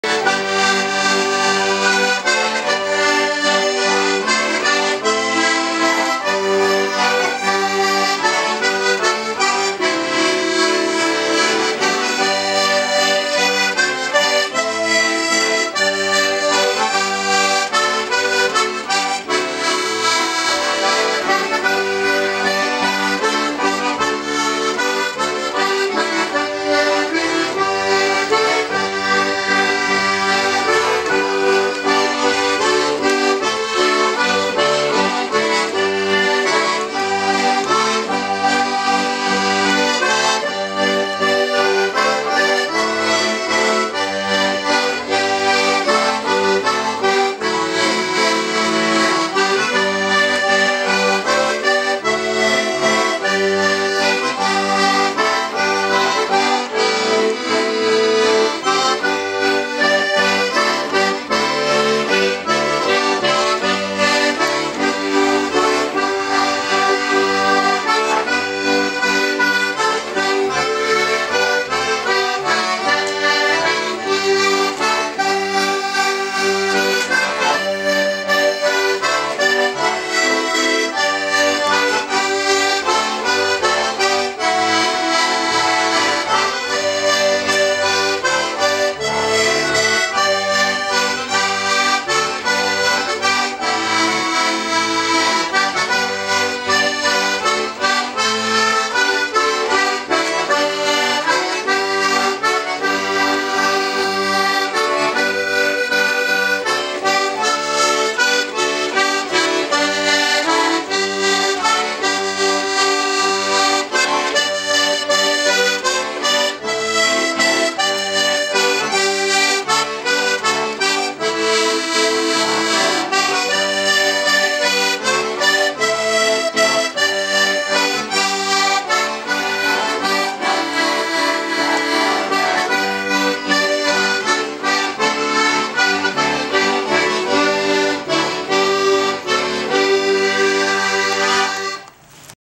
Tribute to Newfoundland Button Accordion Music
I met these two brothers from Bell Island with the Traveling Piano in St. John's Newfoundland, they invited me to their home to jam and spontaneously gift some music. Piano dog Boner came along as always. This is a song by Johnny Cameron.